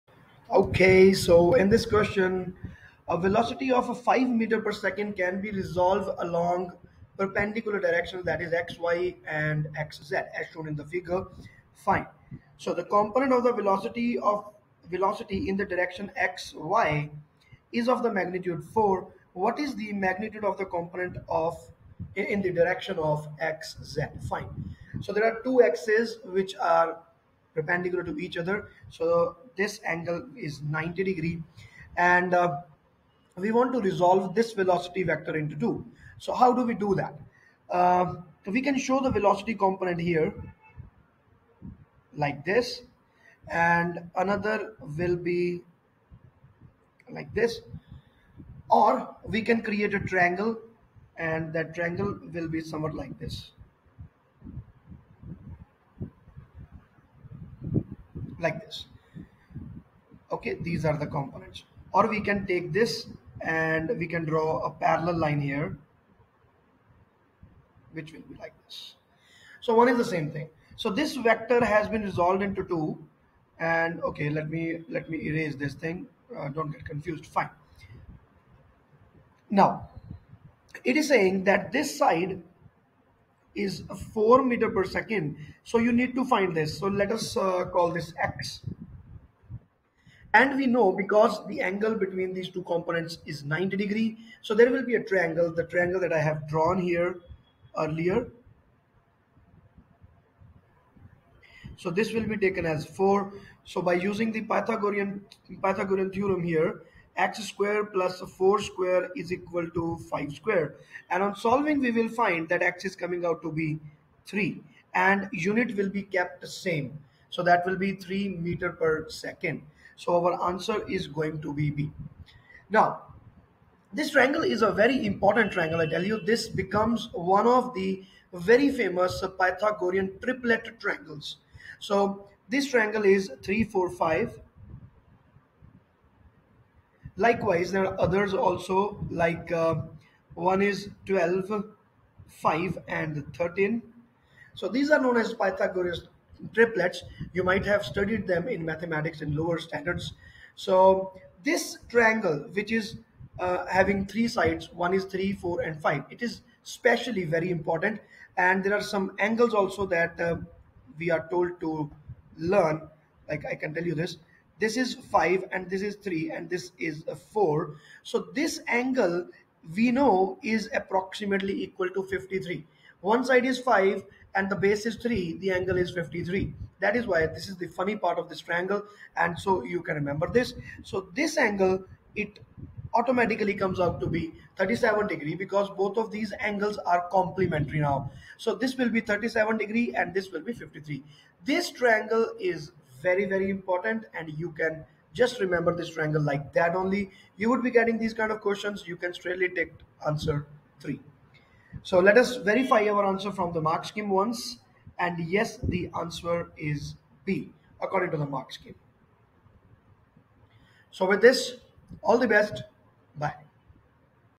A velocity of 5 m s−1 can be resolved along perpendicular directions XY and XZ. The component of the
A velocity of 5 m s−1 can be resolved along perpendicular directions XY and XZ.
The component of the velocity in the direction XY is of magnitude 4 m s−1. What is the magnitude of the component in the direction XZ?

Valid forever.!